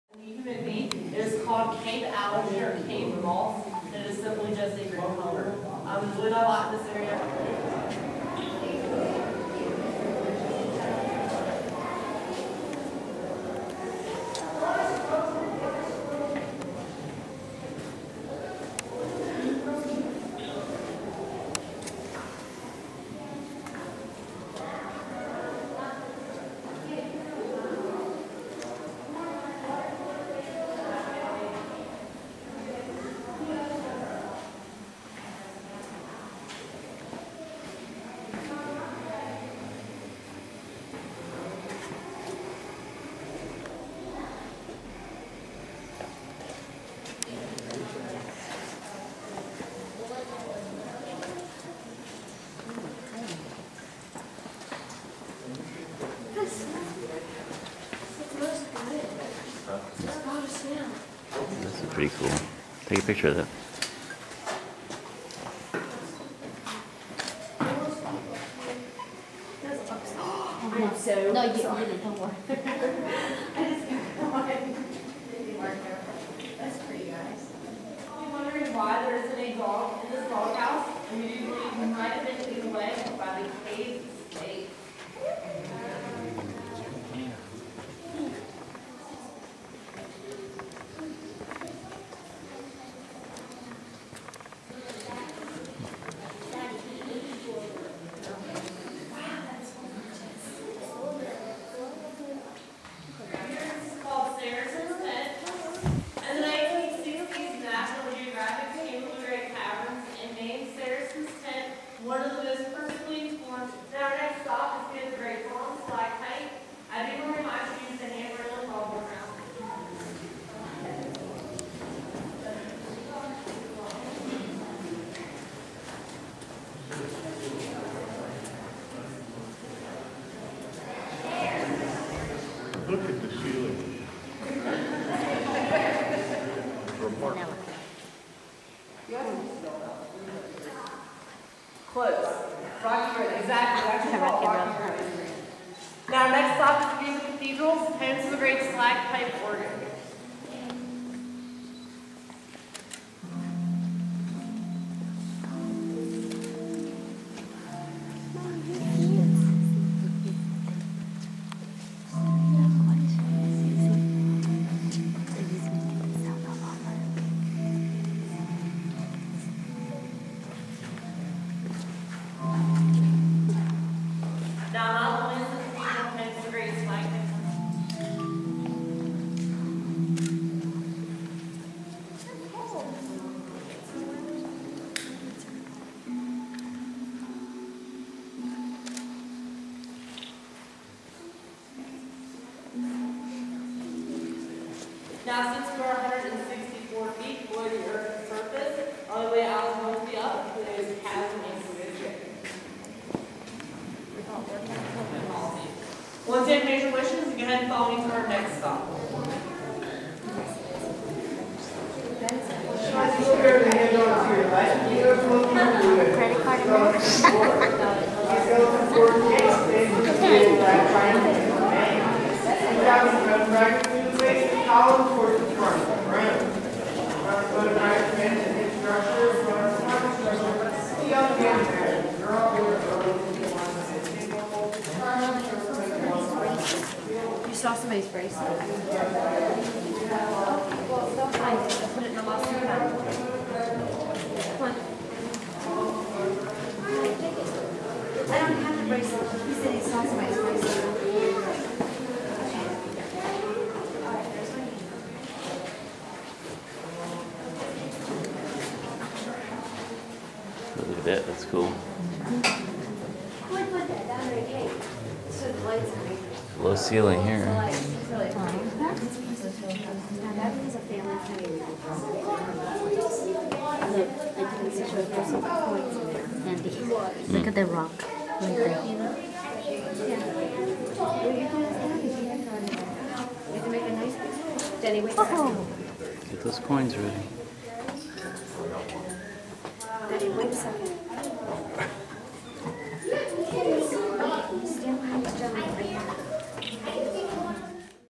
Luray Caverns, Virginia (With Facts/Figures)
I think the last time I visited Luray Caverns was about 40 years ago, when I was a kid, so I was overdue for a return visit. Luray Caverns was discovered in 1878 by a local tinsmith/photographer. It lies beneath the Shenandoah Valley in Virginia. The Caverns attract 500,000 visitors a year and contain thousands of stalactites (formed from the ceiling down) and stalagmites (formed from the floor up). It also contains The Great Stalactpipe Organ, the world's largest musical instrument because the music-playing stalactites cover 3.5 acres.